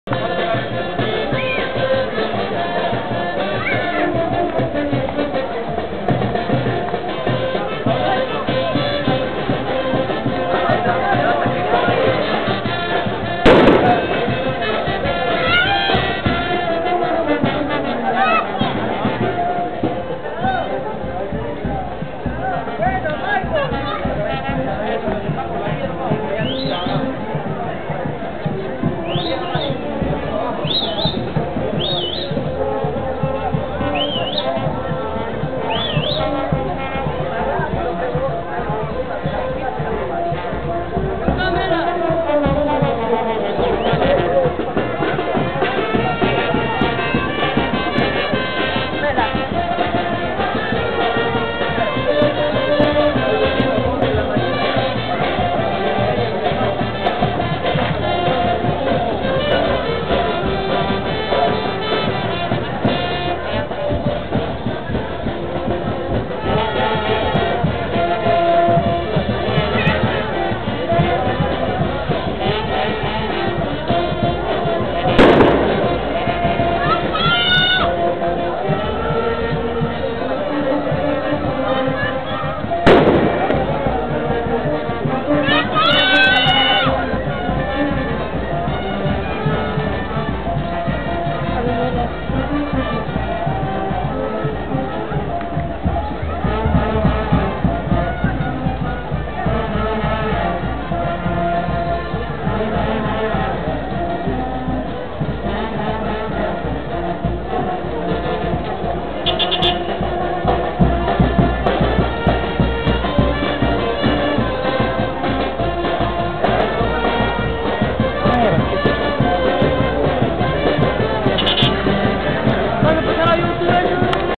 CHILETE 2009 : CARNAVAL
Haciendo su ingreso a la plazuela Jose Galvez toda la mancha carnavalesca, acompañado claro esta de sus respectivas reynas

CHILETE 15-3-09